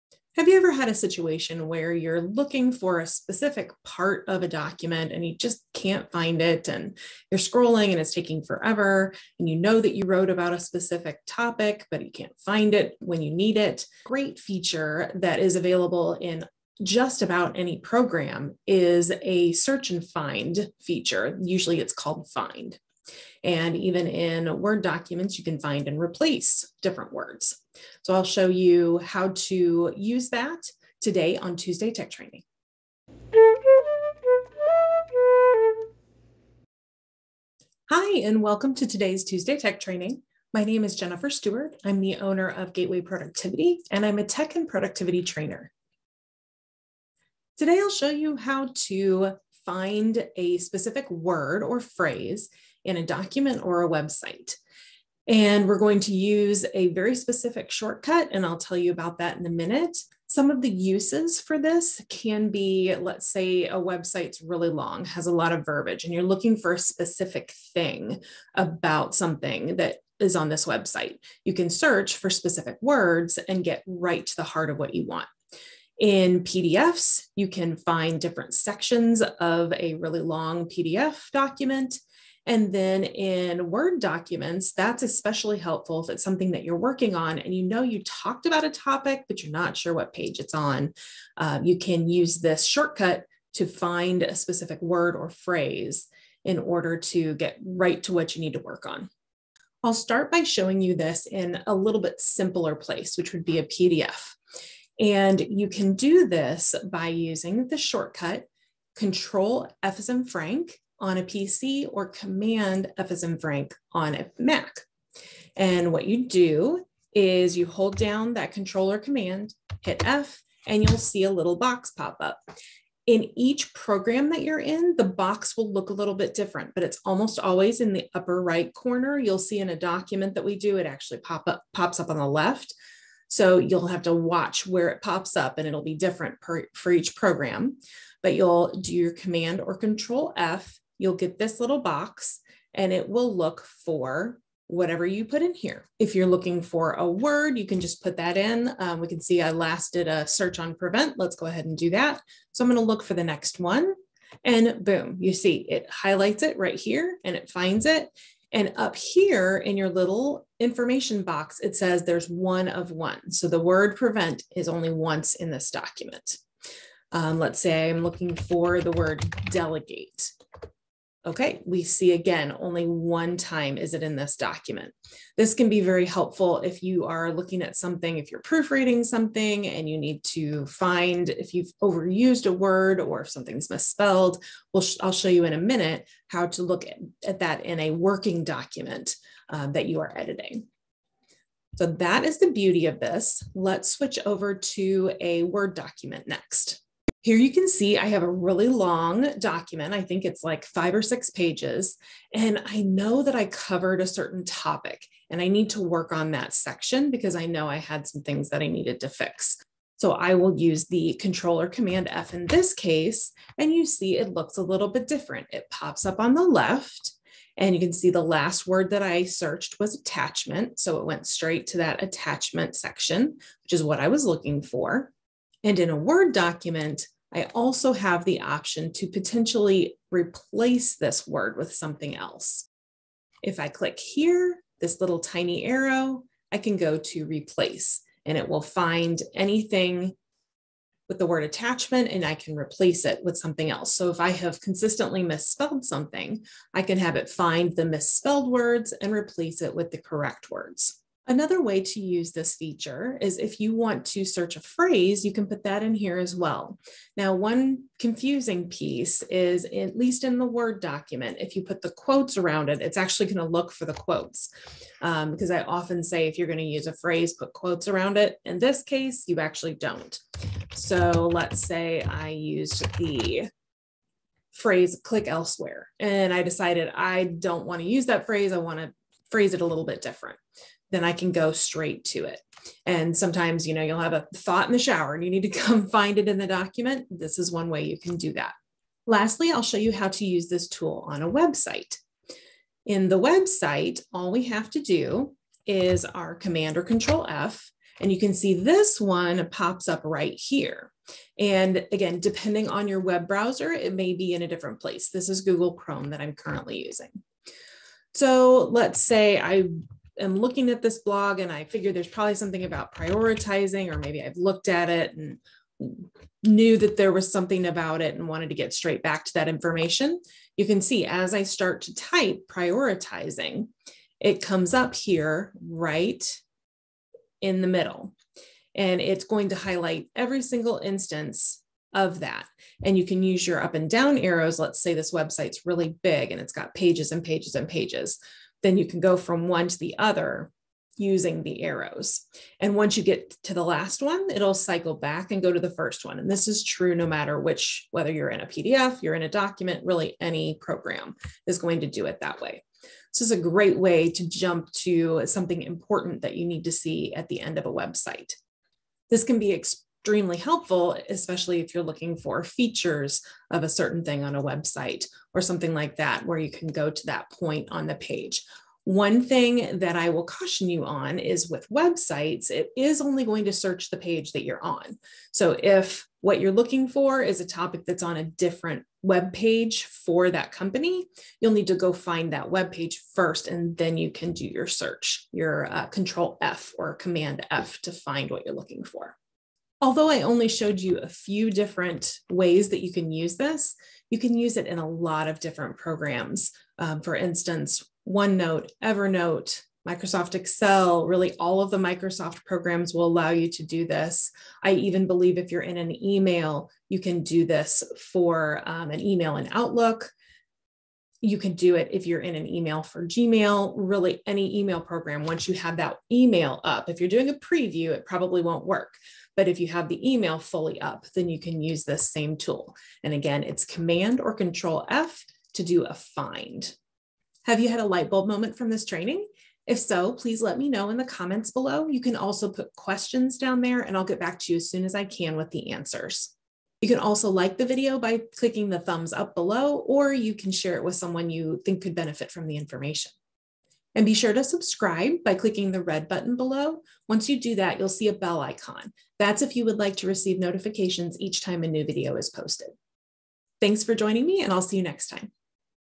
How to Find a Word in a Document or Website
In this Tuesday Tech Training video, learn how to quickly find a word or phrase in a document or web page. The shortcut shown is for both Windows PC and Mac computers.
0:00 - Introduction
1:00 - Examples of Why You Might Use the "Find" Shortcut
1:39 - How to Use the "Find" Shortcut in a PDF
3:17 - How to Use the "Find" Shortcut in a Word Document
3:46 - How to Use Find and Replace in a Microsoft Word Document
4:11 - How to Use the "Find" Shortcut to find a Phrase
4:49 - How to Use the "Find" Shortcut on a Website
6:29 - Additional Examples of Programs Where You Can Use the "Find" Shortcut

Windows users:
o Ctrl + F to find a word or phrase in a program or website

Mac users:
o Command + F to find a word or phrase in a program or website